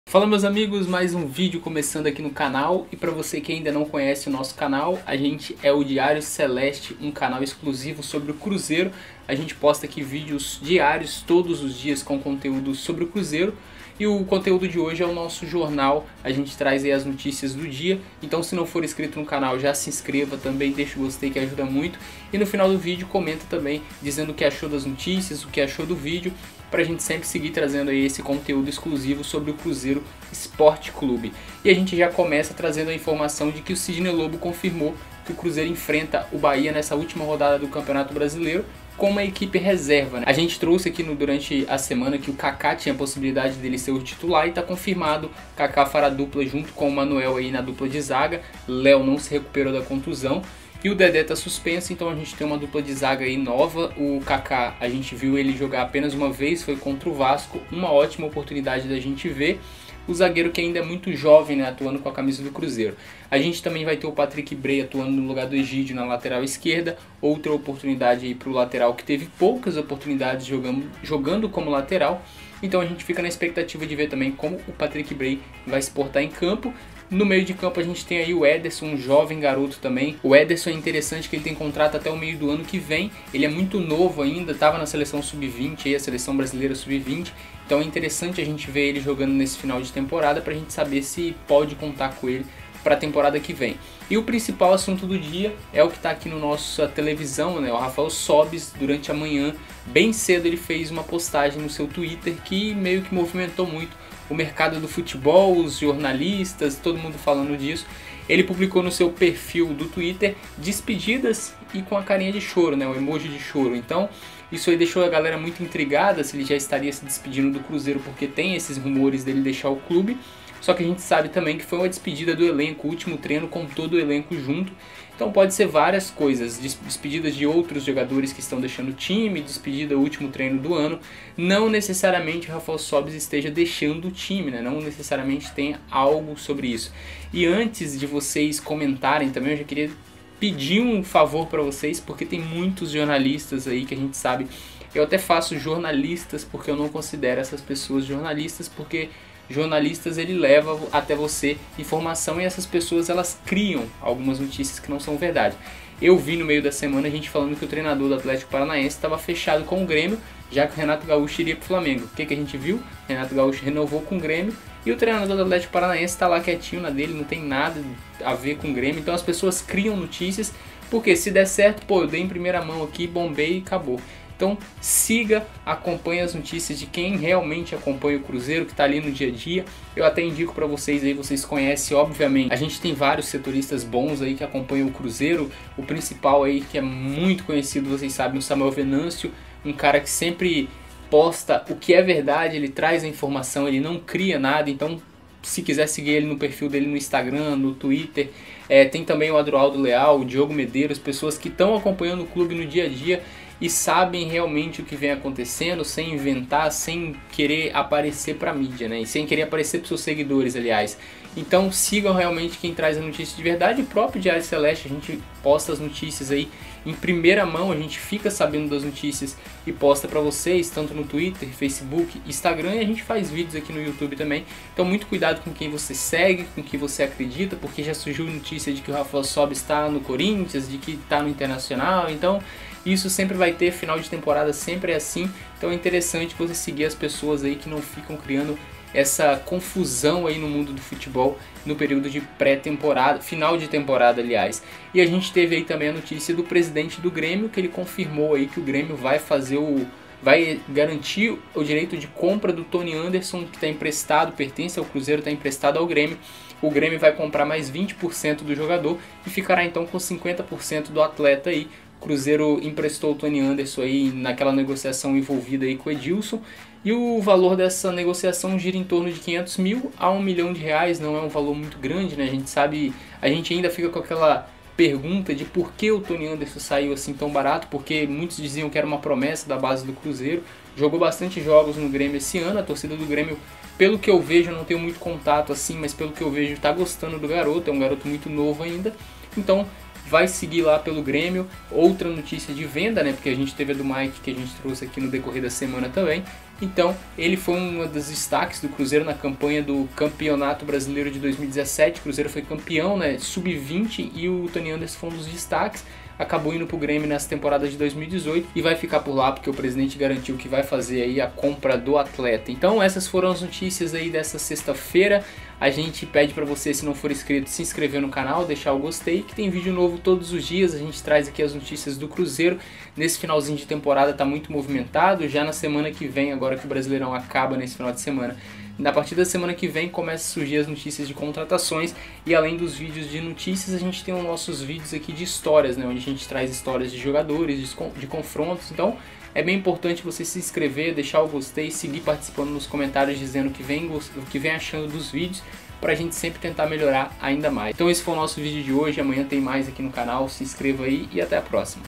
RAFAEL SÓBIS DE SAÍDA? - DIA CELESTE
O Rafael Sóbis fez uma postagem hoje em seu perfil no Twitter que deixou todo mundo curioso, e surgiu várias notícias que o atacante estaria deixando o Cruzeiro, será que é verdade? Assista nosso vídeo com todas as informações.